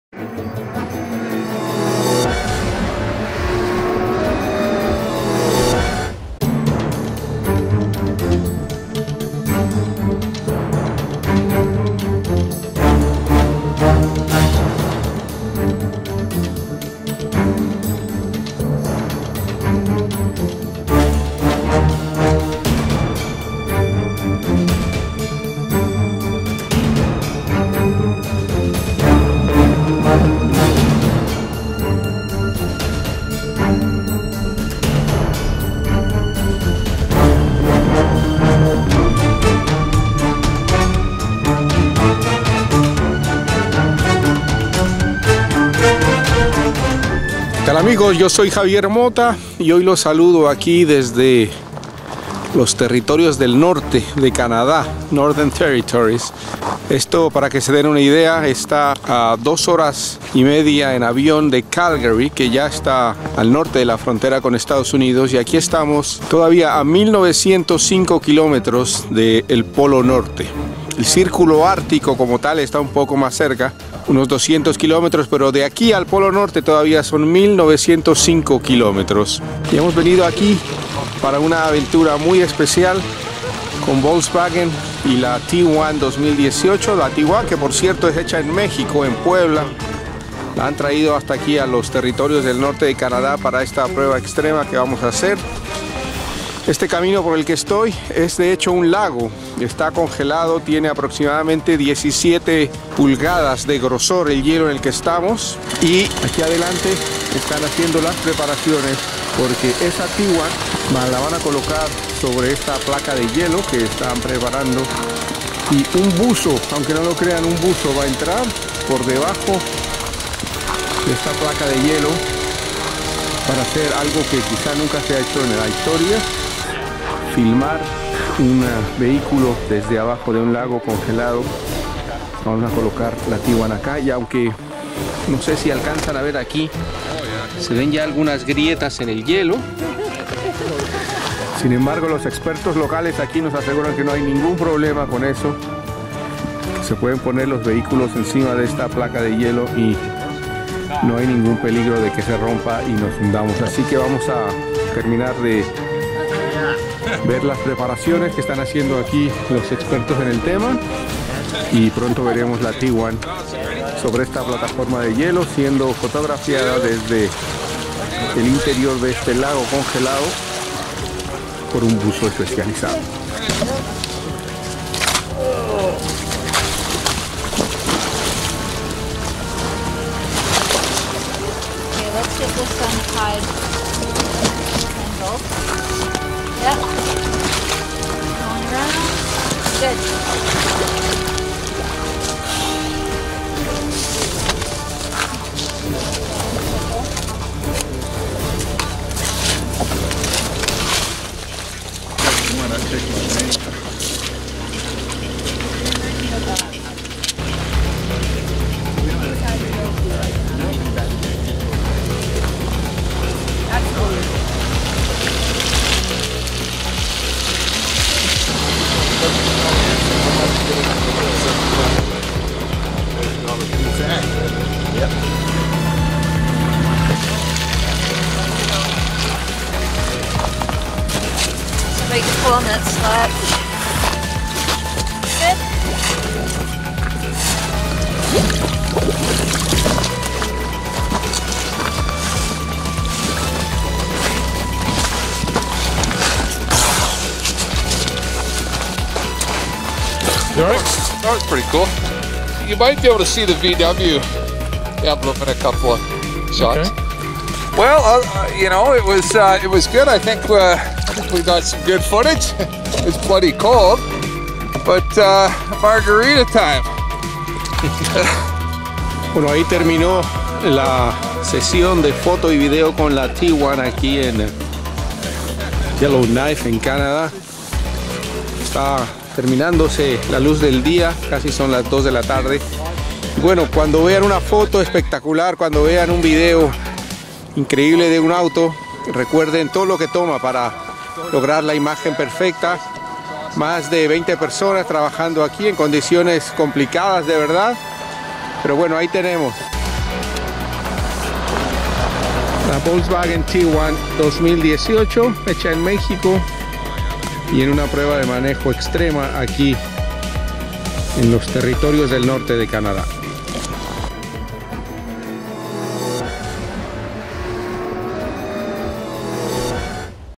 Volkswagen Tiguan 2018 en photo shoot extremo en Canadá
Volkswagen Tiguan 2018 en photo shoot extremo en Canadá. Un buzo se metió a un lago congelado para capturar la imagen perfecta de esta SUV MADE in MEXICO